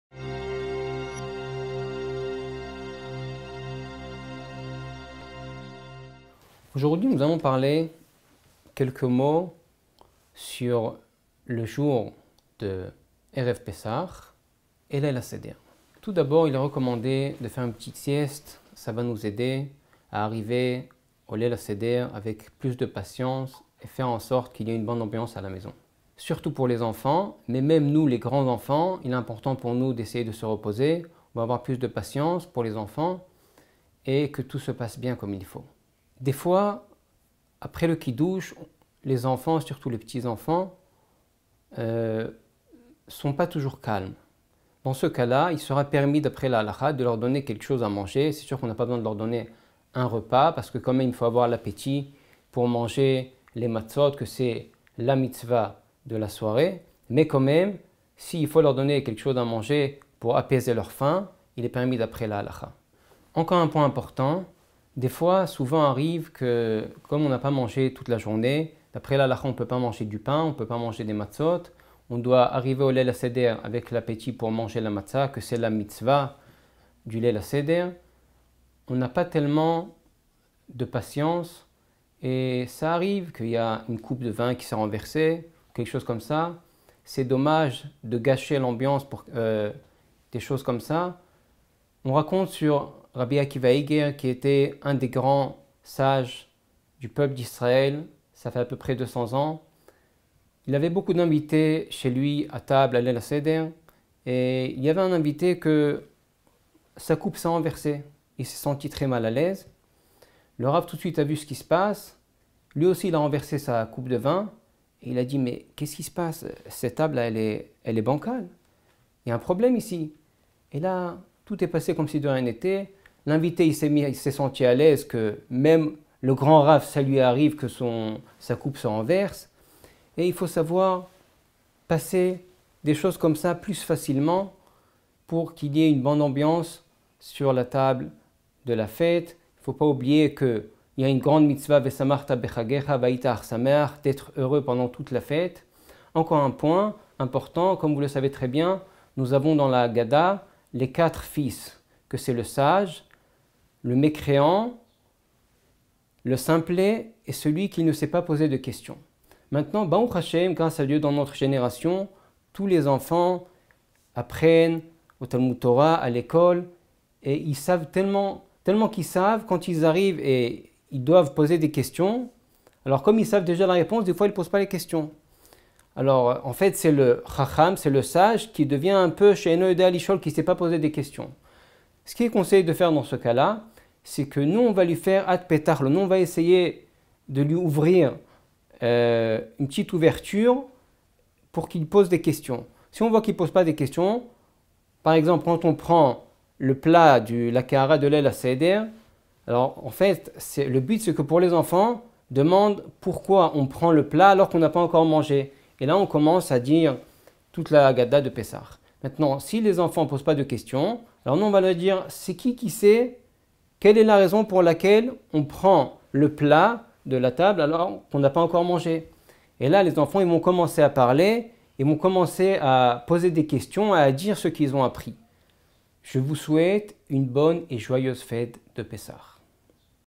A l’approche de Pessah - Rav Chlomo Nadav
Comment se préparer au premier soir de pessah?